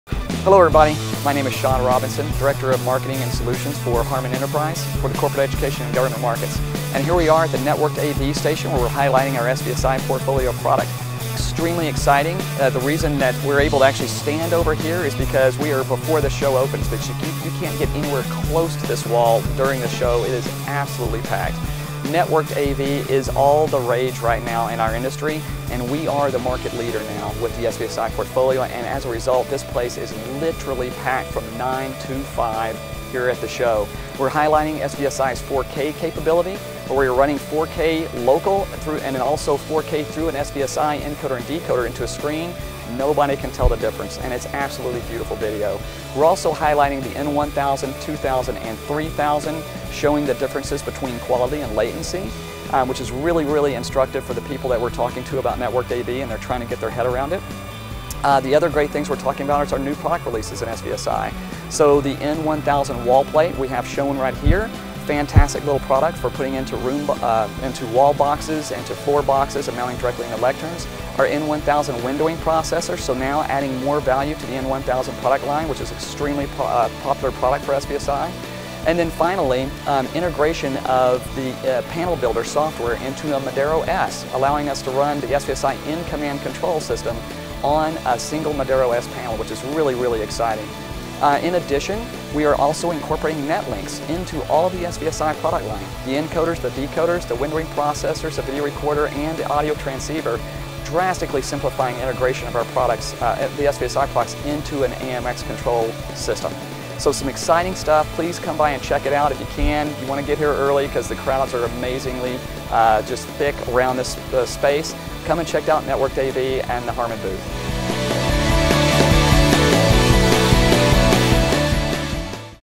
InfoComm 2016: Meet HARMAN's AMX SVSI Networked AV Solutions
Shaun Robinson, Director of HARMAN Professional Enterprise Solutions takes you on a tour of all the AMX SVSI Networked AV solutions being demonstrated at the show this year.